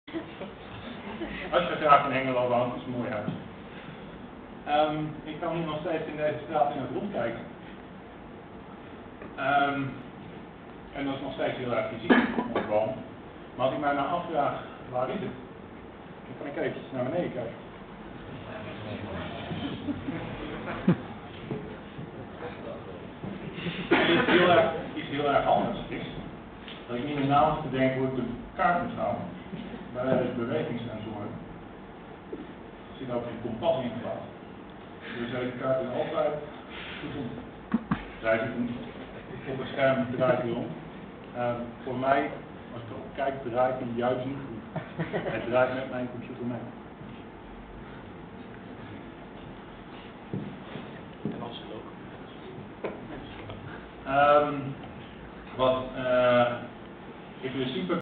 WIImote bewegingssensor toepassing
Aldo Houben toont hoe je bewegingssensoren ook kunt toepassen aan de hand van 3d-foto's